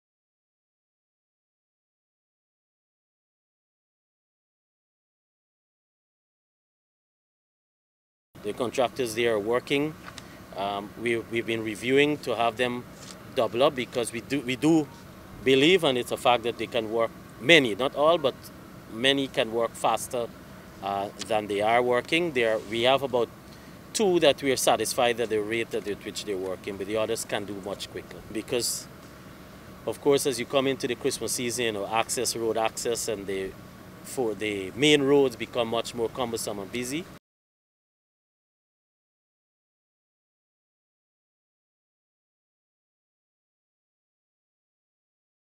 CONTRACTORS NEED TO SPEED UP WORK ON SCHOONORD TO CRANE FOUR-LANE HIGHWAY – CROAL
*** Works currently being executed on the Schoonord to Crane four-lane highway are under review and to ensure that the project is completed in time, there is a need for some contractors to work faster. And to do this, they may need to ‘double up’, Minister of Housing and Water, Collin Croal said on Thursday. Croal made the disclosure during a visit to the West Coast of Demerara to inspect ongoing infrastructural works. The $11.8 billion contract for the highway was awarded to eight contractors. Croal said he is only satisfied with the progress made by two of the contractors thus far. The highway from Schoonord, West Bank Demerara to Crane, West Coast Demerara is expected to be completed by June 2024. The new highway complements ongoing infrastructural works within the region especially the development of new housing areas. When completed, it is expected to significantly reduce traffic congestion on the west side.  The project will see the construction of 4.1 kilometres of a dual carriageway reinforced concrete road with an emergency lane as well as 2.4 kilometres of road rehabilitation, and upgrade. It will also feature two roundabouts, 11 reinforced concrete box culverts, 36 pre-stressed bridges, and road signage and markings.